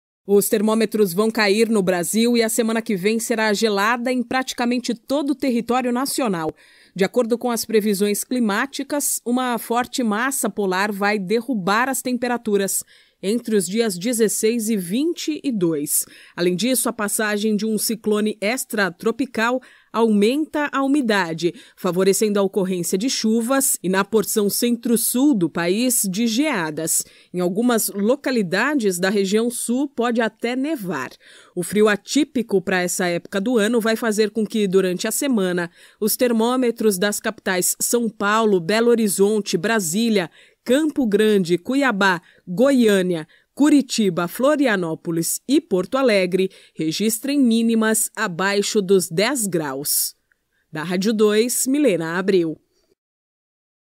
ONDA DE FRIO PROMETE DERRUBAR AS TEMPERATURAS NO BRASIL NA SEMANA QUE VEM
| BRASIL |